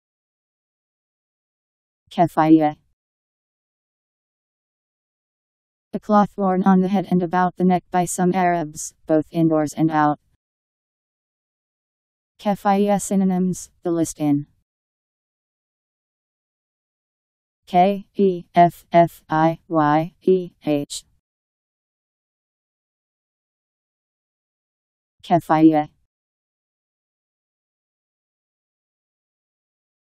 Keffiyeh Meaning
Video shows what keffiyeh means. A cloth worn on the head and about the neck by some Arabs, both indoors and out.. keffiyeh synonyms: the list in.  Keffiyeh Meaning. How to pronounce, definition audio dictionary. How to say keffiyeh. Powered by MaryTTS, Wiktionary